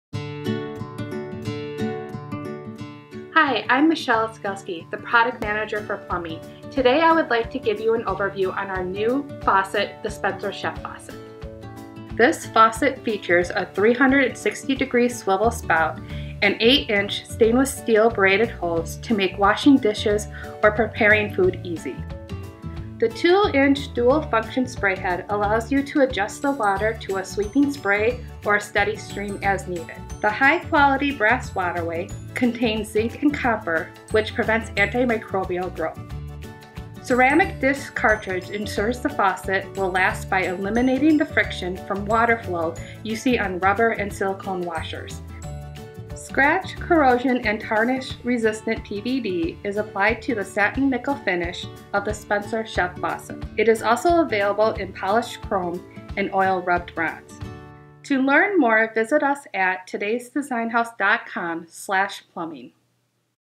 Design House Spencer Chef Kitchen Faucet – Product Overview
The Spencer Chef Kitchen Faucet makes washing dishes or preparing food easy with its 360-degree swivel spout and 8” reaching hose. The 2” dual function spray allows you to adjust the water to a sweeping spray or a steady stream as needed. The high-quality brass waterway contains zinc and copper which prevents antimicrobial growth. Ceramic disc cartridge ensures the faucet will last. This faucet is CUPC, Lead Free, and ADA compliant.